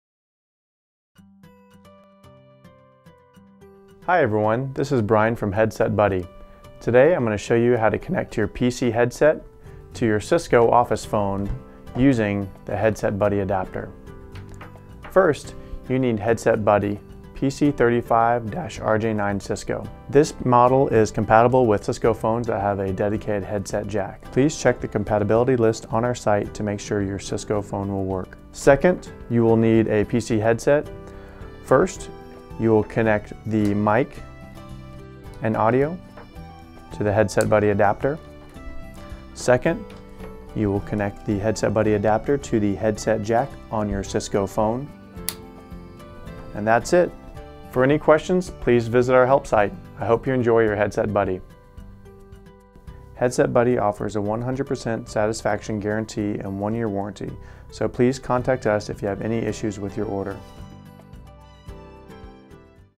How to Connect Your PC Headset to Cisco Phone Using Headset Buddy
- How to Connect Your PC headset to Cisco Phone Using Headset Buddy Adapter PC35RJ9Cisco

Why would you buy a different headset for every device and telephone you own when there is a Headset Buddy for almost every device you could need.

This video will show you how you can use the PC headset that you already have available and connect it to your Cisco telephone using a simple Headset Buddy adapter.

Long Tail Products
3005 S. Lamar
Suite D-109 #146